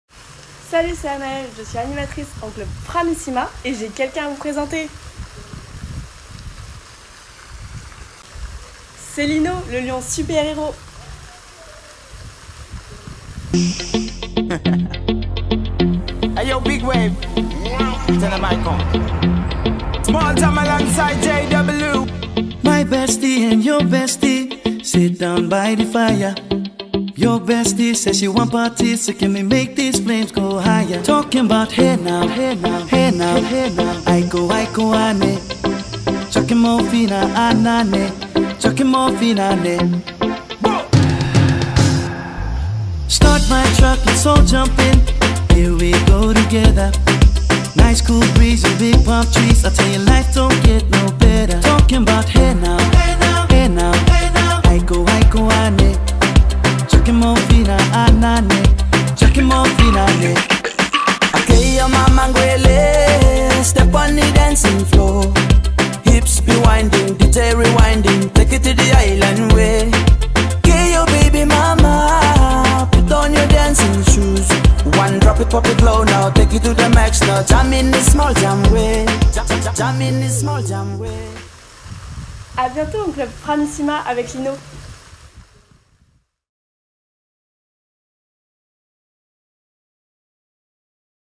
Présentation Lino, la mascotte des clubs Framissima [Voyages FRAM officiel]
Lino, le lion super-héros, est la mascotte des Clubs Enfants en hôtels Clubs Framissima !
Il participe à de nombreux événements festifs pour votre plus grand plaisir !

Dès cet été, grâce à ses supers pouvoirs et sa cape magique, Lino vole de club en club à travers la Grèce qui ne compte pas moins de 15 clubs Framissima.